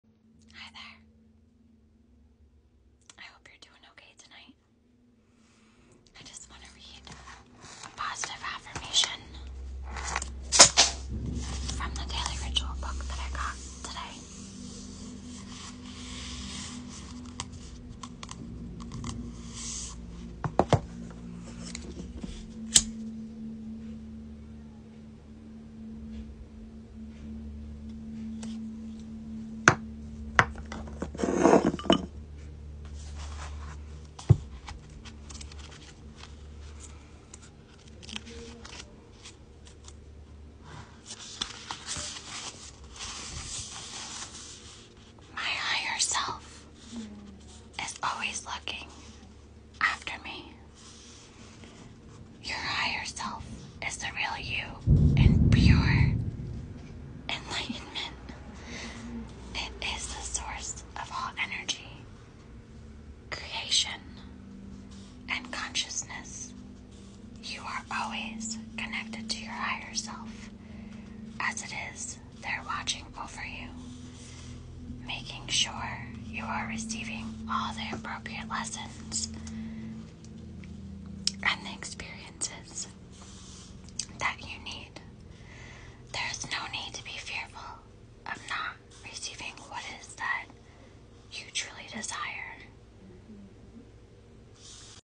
Positive affirmation same
Reading a affirmation  from a book that that is titled Daily Rituals.